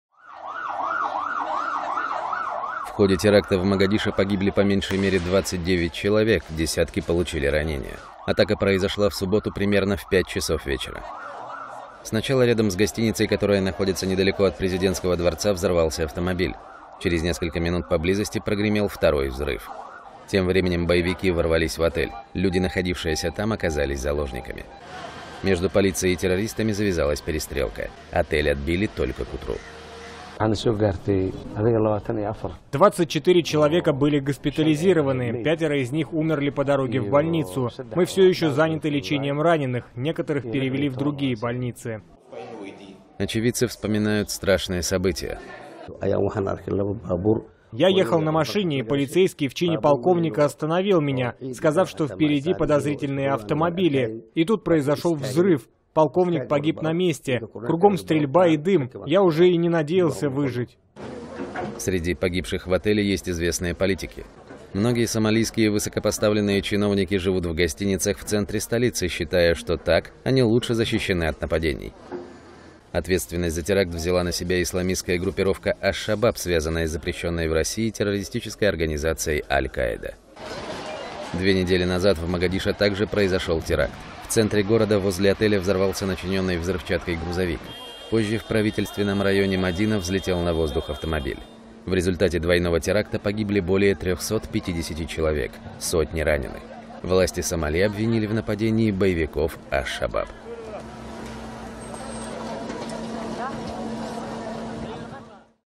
Очевидцы теракта в Могадишо вспоминают о пережитом (новости)
По меньшей мере, 29 человек погибли в результате нападения на отель в столице Сомали - Могадишо. Теракт произошёл в субботу. Ответственность взяла на себя исламистская группировка «Аш-Шабаб».